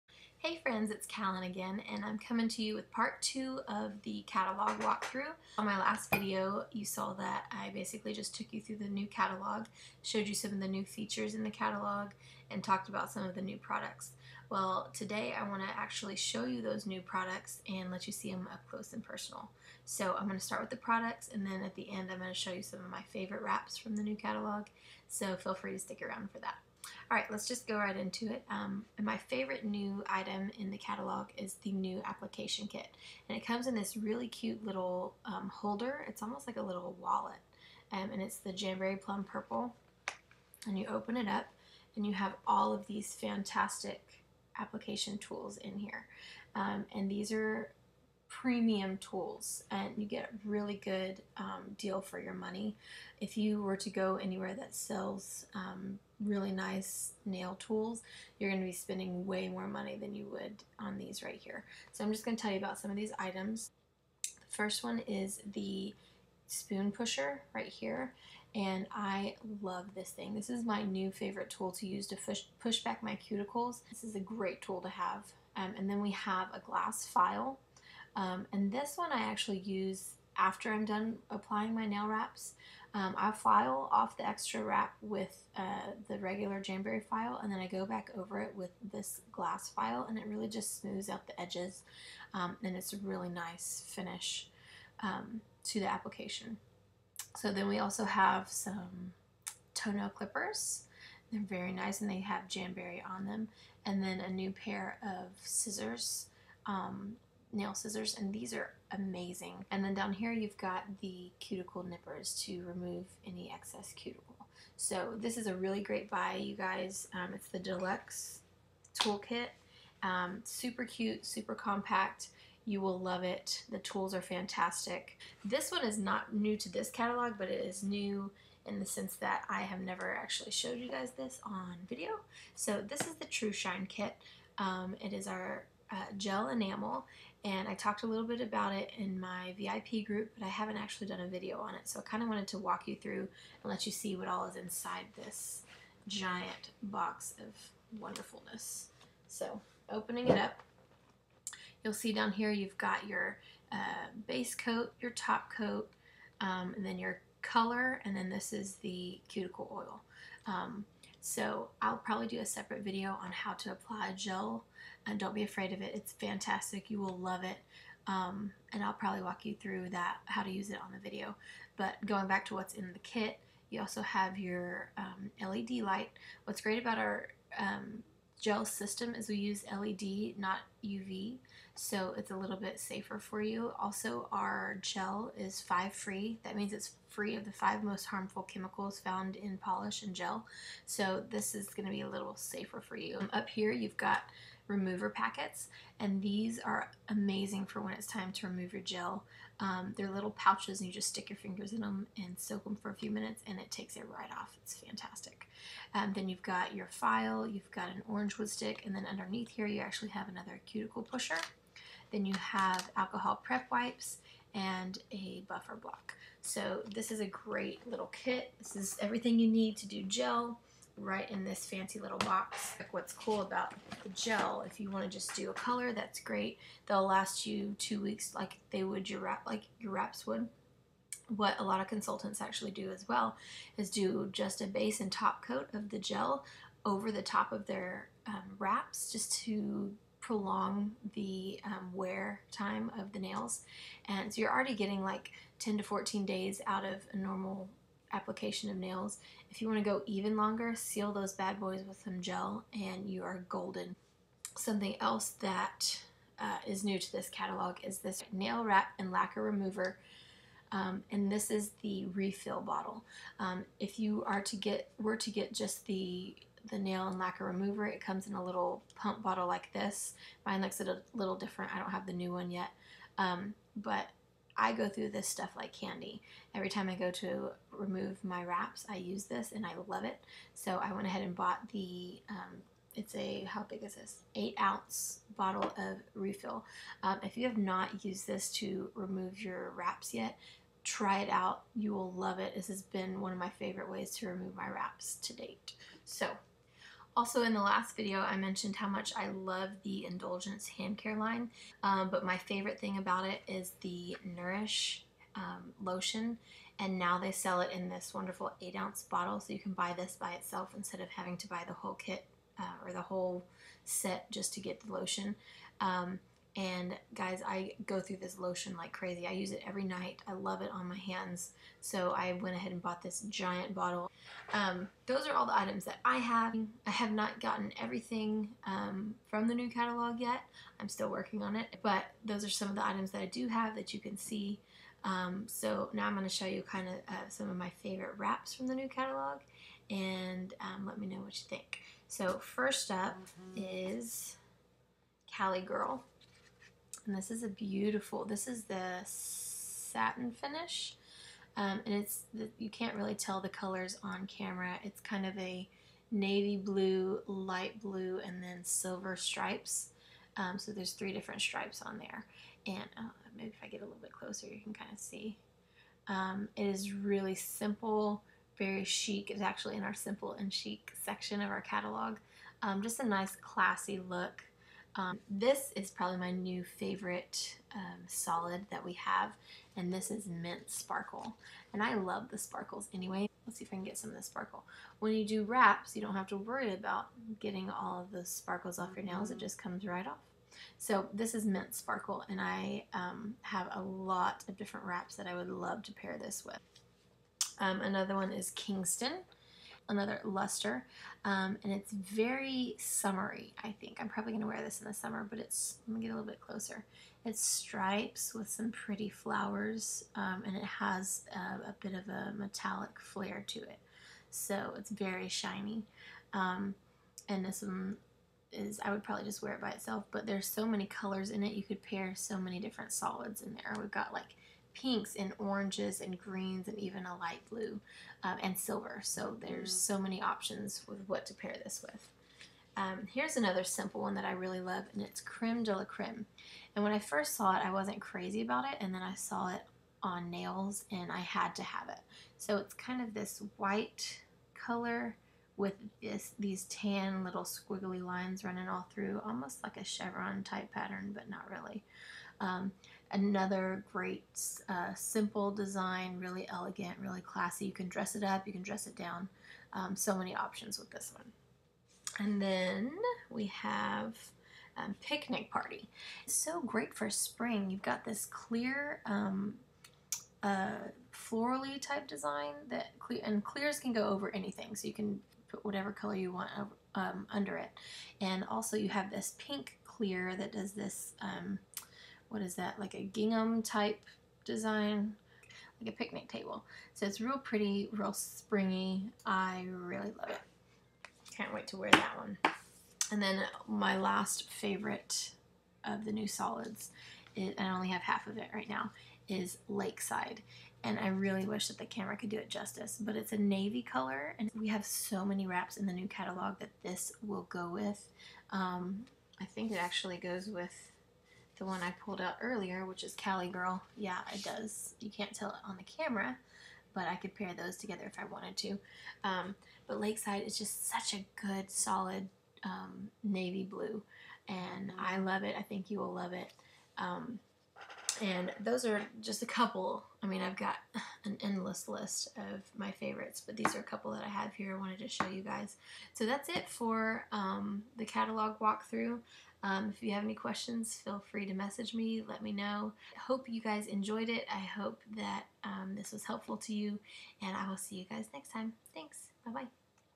My Favorite Items from the New Catalog
Check out some of my favorite items from the new catalog up close and personal! Then stick around to see my favorite wraps and what to pair them with!